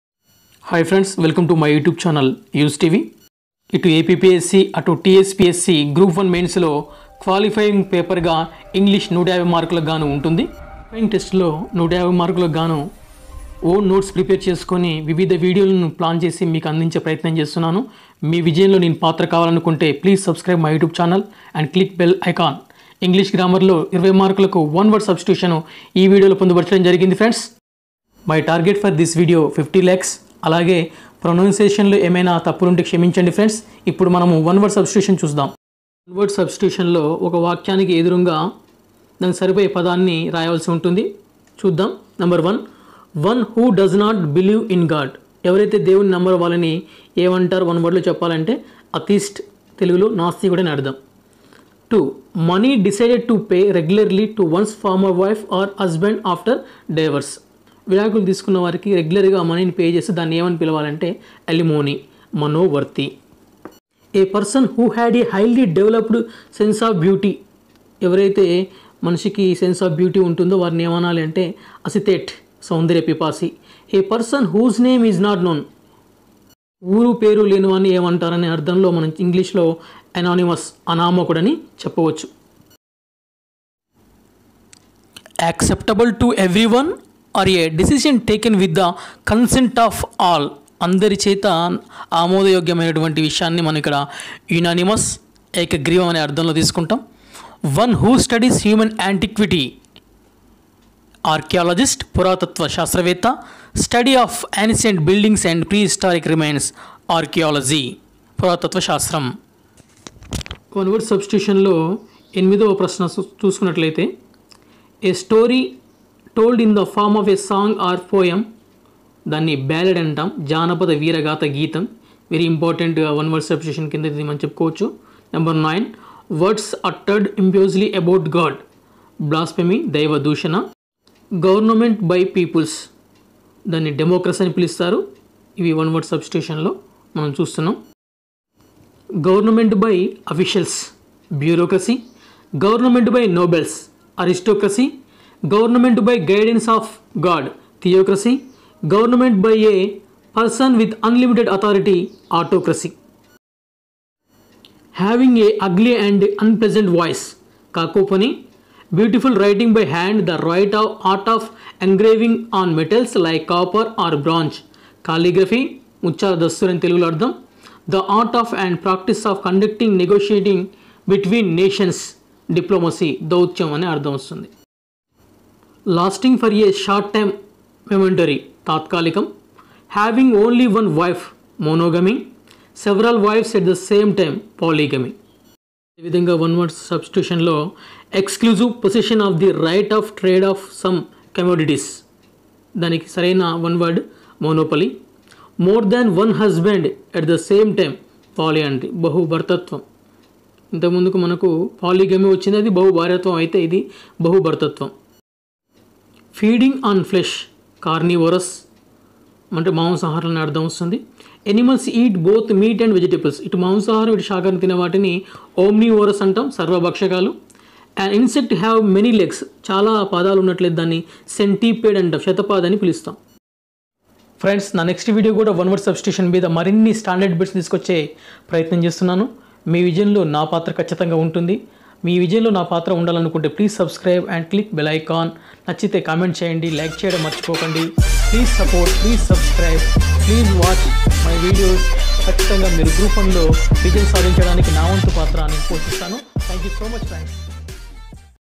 General English one word substituions for ap grama,ward sachivalayam jobs
english one-word-substitutions
ap_grama sachivalayam jobs#ap#sachivalsysm#notifivstion
General English one word substituions for ap grama,ward sachivalayam jobs#Group1 mains appsc/Tspsc
20marks for English grammar in appsc mains English paper.its a qualifying paper .